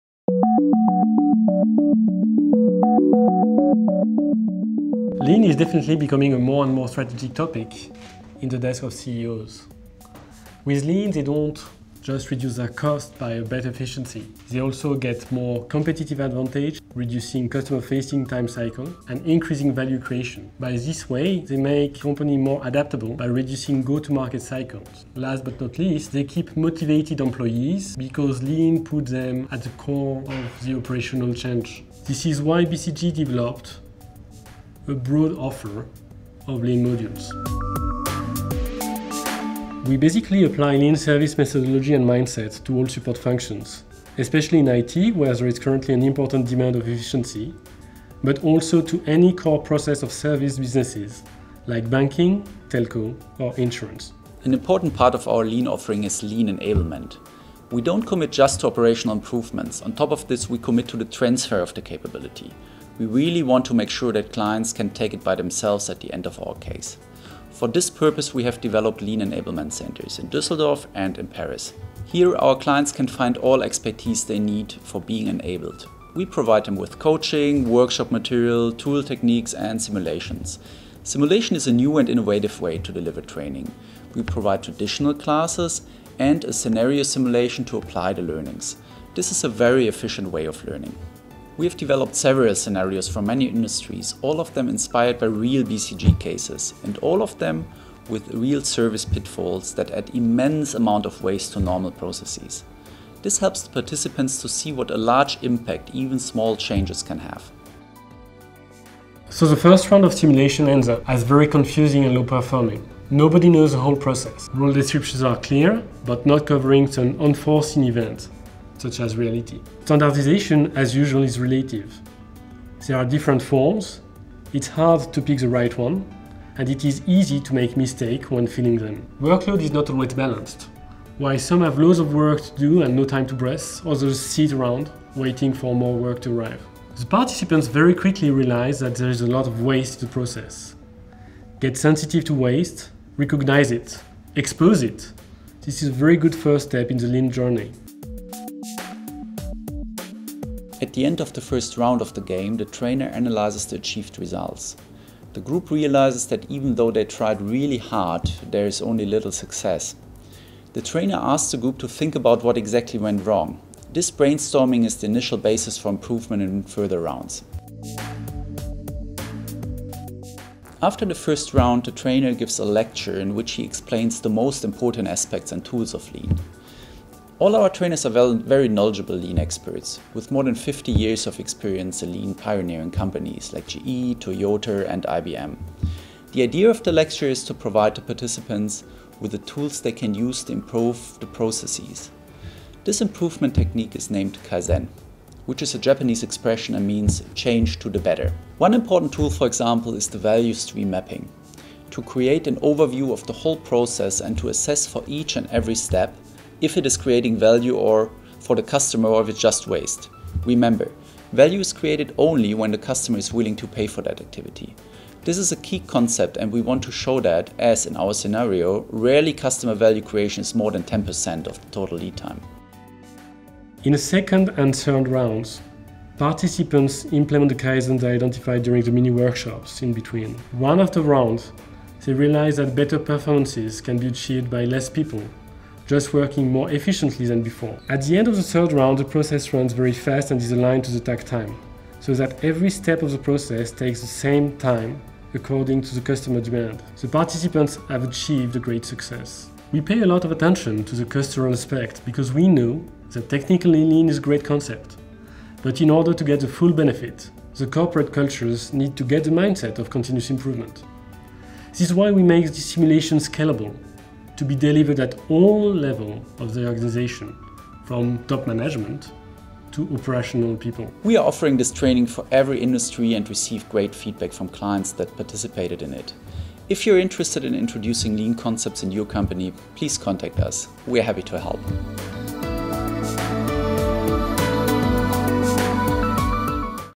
BCG's Nicolas Harlé and Olaf Rehse on Lean Services Enablement
Nicolas Harlé and Olaf Rehse discuss BCG's Lean Services Enablement Center and explain how BCG transfers lean methodology, capabilities and mindset to clients through interactive lean simulation trainings.